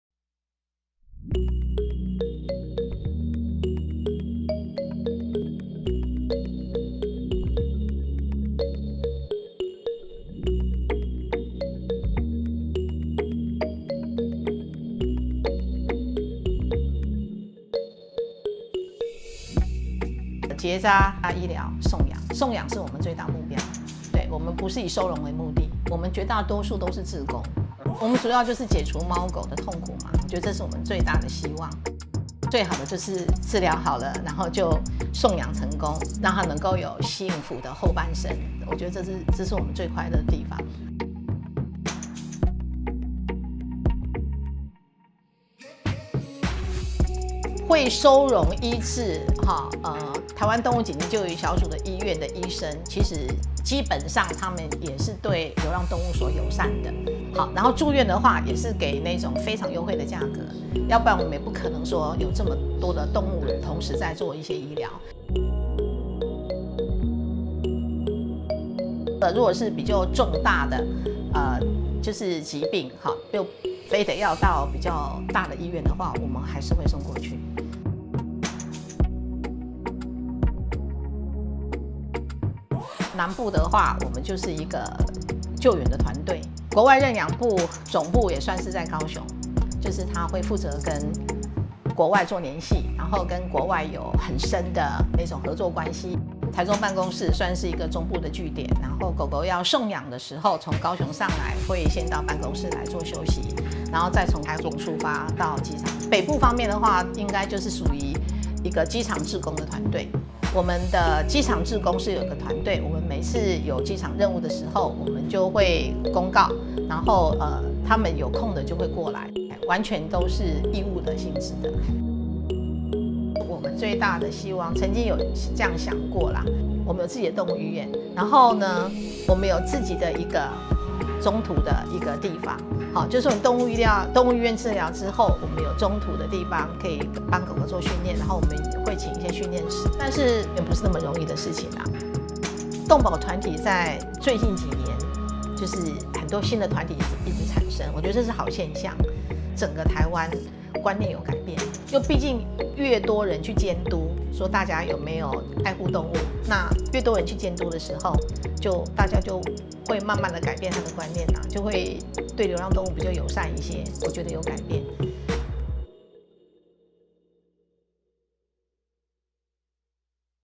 【優報導—ARTT 臺灣動物緊急救援小組 專訪】上山下海只為營救意外流浪動物 尊重守護共生共存的每條生命
一個國家、一座城市的進步，應是能夠包容尊重所有生活其中的生命！在
共有共享的空間一起生活，儘管只是一頓飽飯也不吝於伸出援手幫助；沒有
餘力也別傷害牠們，我們最大的希望與期盼，是臺灣社會能夠接納這些流浪
動物！
臺灣動物緊急救援小組 Animal Rescue Team TAIWAN (ARTT) ，1995 年
由倪兆成先生於高雄創立，是臺灣第一個專門為意外傷病、受虐與流浪動物
提供緊急救援醫療的民間組織。秉持尊重每個生命的堅定信念，從一開始的
獨自奔走援救，到如今志工遍佈各地，20 多年來完成過無數次救援任務、
營救近5 千隻傷病貓狗，更透過「貓狗聯合勸募平臺」幫助弱勢貓狗園，每
個月的飼料支出超過300 萬元，間接幫助超過2 萬個弱勢生命。